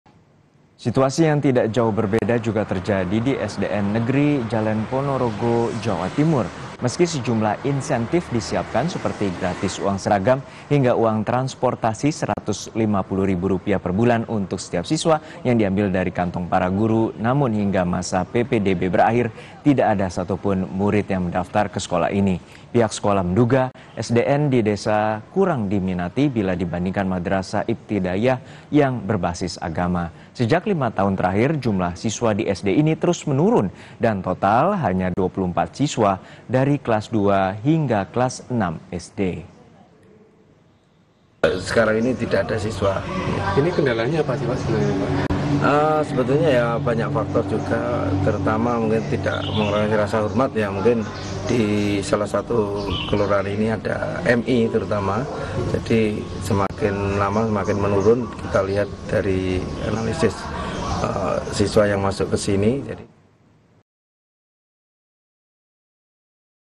Momen Hari Pertama Masuk Sekolah, SD Negeri di Ponorogo Jawa Timur Tak Mendapatkan Murid!
Ponorogo, Kompas.TV Jawa Timur - Hari pertama masuk sekolah, sebuah SD Negeri di Ponorogo, Jawa Timur tak mendapatkan siswa. Meski berbagai jurus dikerahkan sekolah untuk mendapatkan siswa baru, namun hingga PPDB berakhir tak ada satu pun siswa baru yang bersekolah disini.